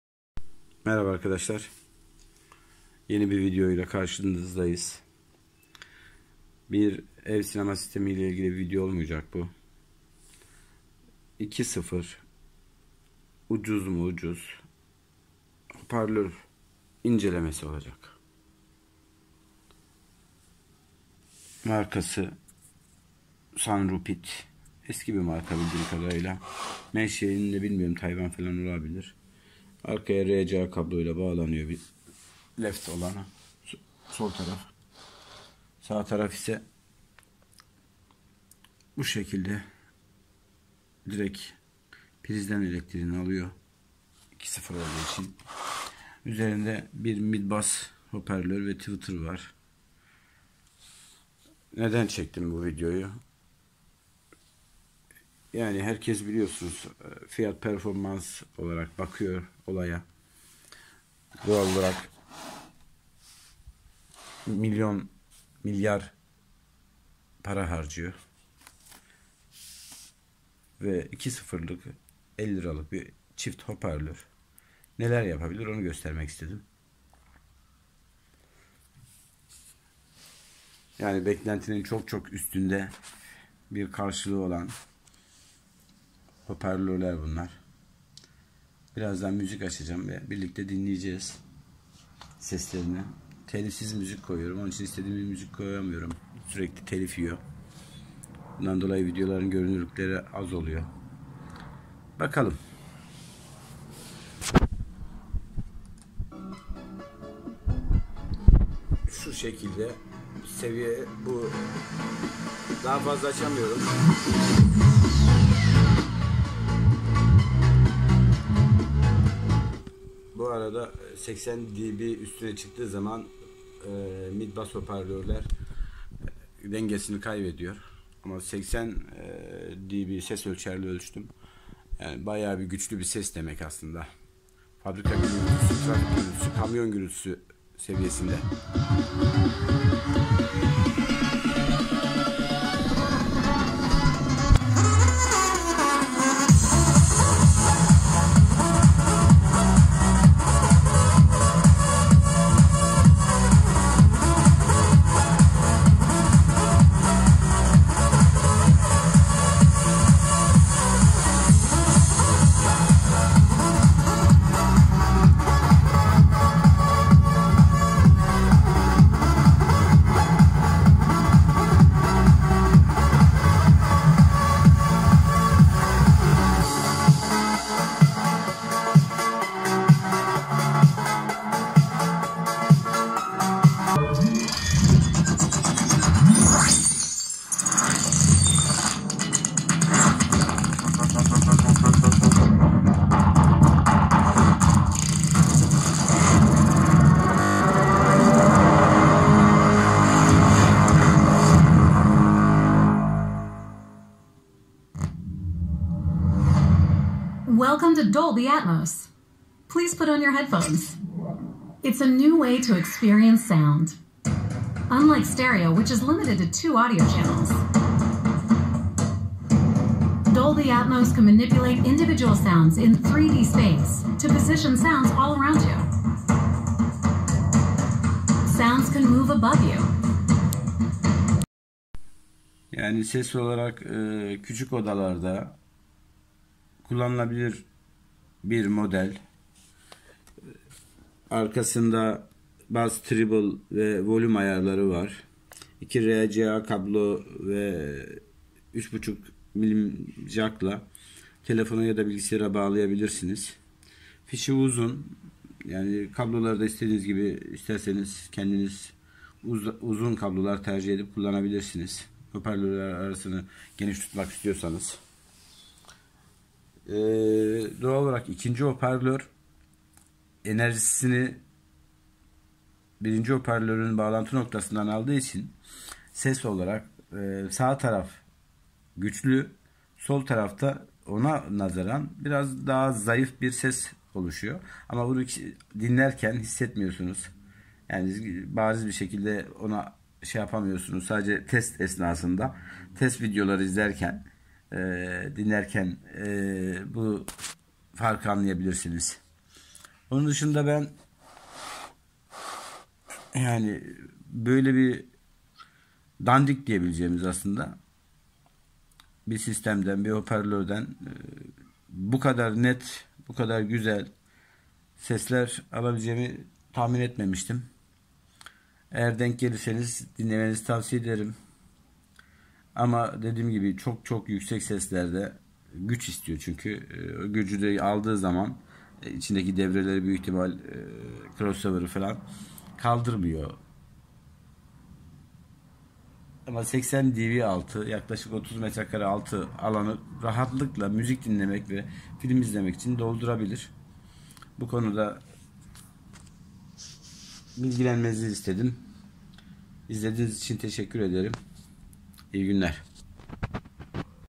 Sanrupid sr 60a 2.0 speakers
Sanrupid sr 60a 2.0 hoparlör incelemesi.
Gayet başarılı fiyat performans canavarı.
30 watt streo hoparlörler.
Pc vb bağlantı ile kullanmaya uygun. Cihazın arkasında on/of, bass, trible, volume ayarları için düğmeler var, bağlantı 2 rca ve 3.5 mm jack ile yapılıyor, hoparlör ise tek rca kablo ve 3.5 mm jack kablo ile sağlanıyor. Ses yönünden tatminkar. (küçük alanlarda ve müzik ağırlıklı dinlemeye uygun, film izlemek için başlangıç için hayli hayli yeterli.
Sinema etkisi tabi ki yok.
İyi seyirler.